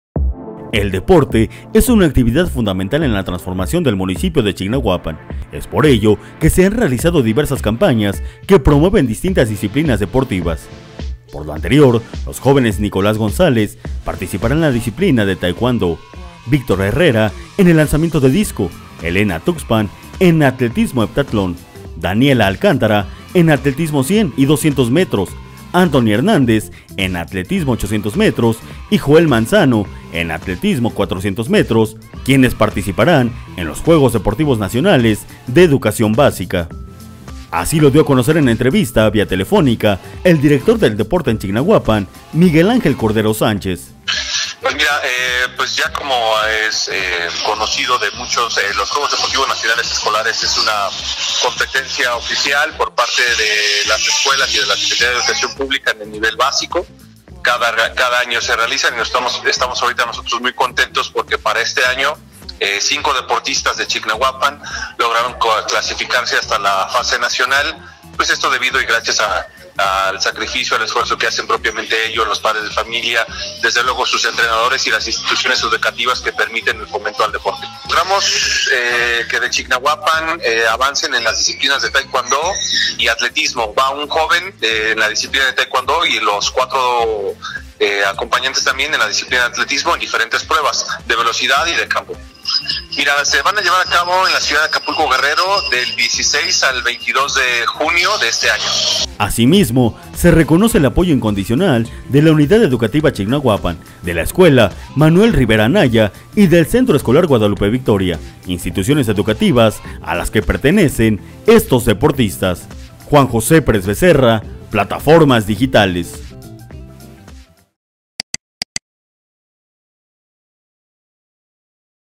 Chignahuapan presente en Juegos Deportivos de Educación Básica en Acapulco
Jóvenes del municipio de Chignahuapan participarán en las disciplinas de Taekwondo y Atletismo de los JuegosDeportivos Nacionales de EducaciónBásica e celebrarse en Acapulco, Guerrero